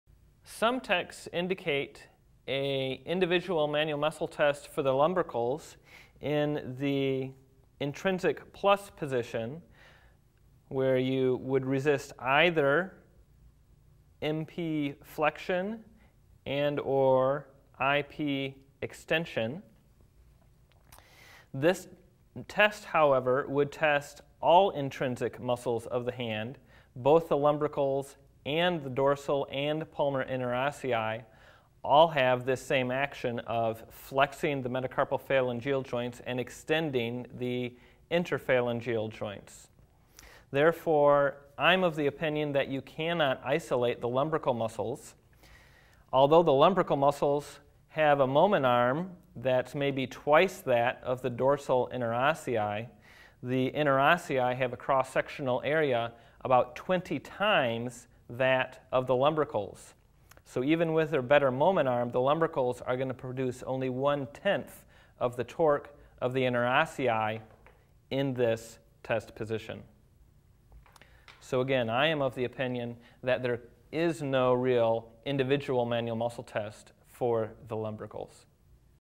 MMT Individual Muscle Lumbricals (for what it is worth...)
How to evaluate lumbrical strength with Manual Muscle Testing (MMT).
I do not think this test isolates the lumbricals, but I include it because some texts mention it.